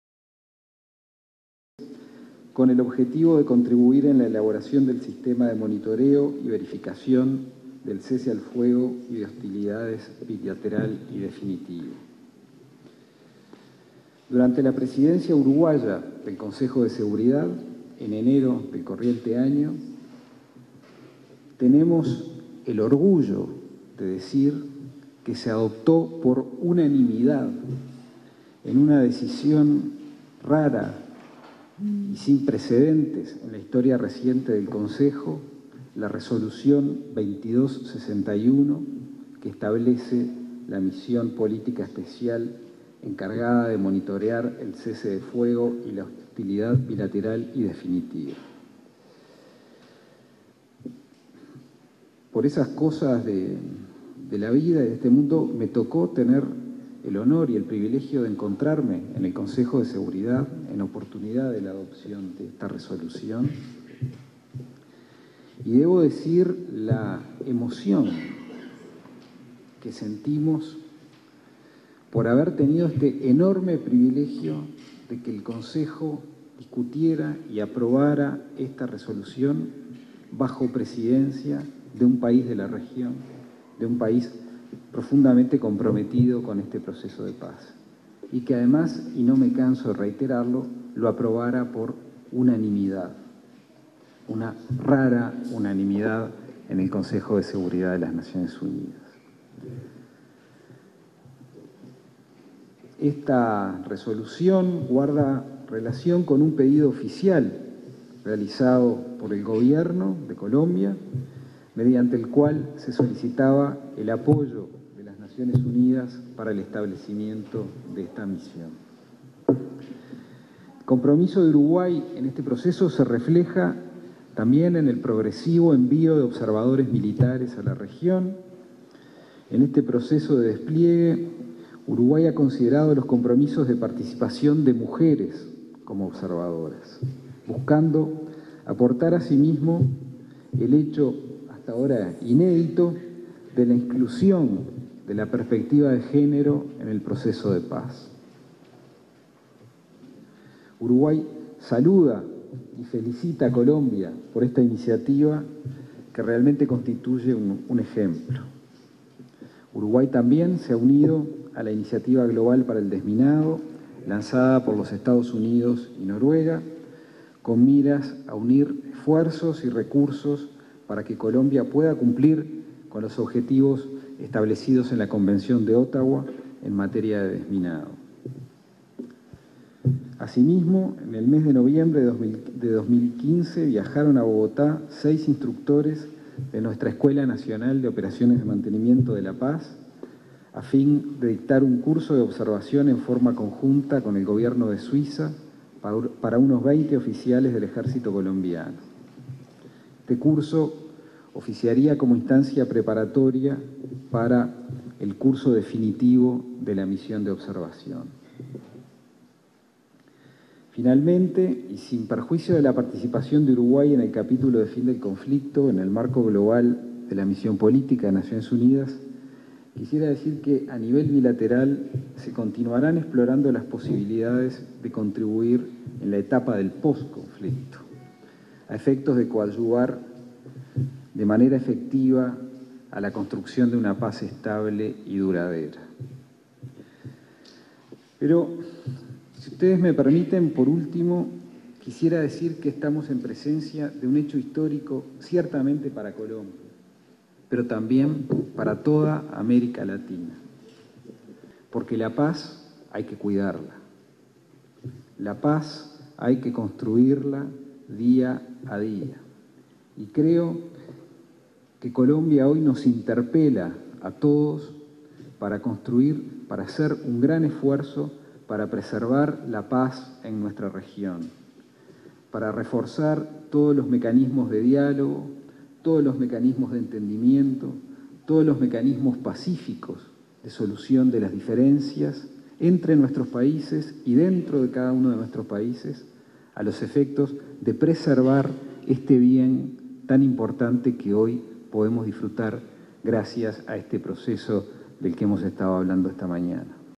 Tras acuerdo en Colombia con las FARC, América es una zona de paz.
El subsecretario José Luis Cancela destacó la importancia de la llegada de la paz a Colombia que convierte al continente en una zona de paz. El jerarca habló de la importancia que para Uruguay tiene este hecho, como integrante del Consejo de Seguridad de Naciones Unidas.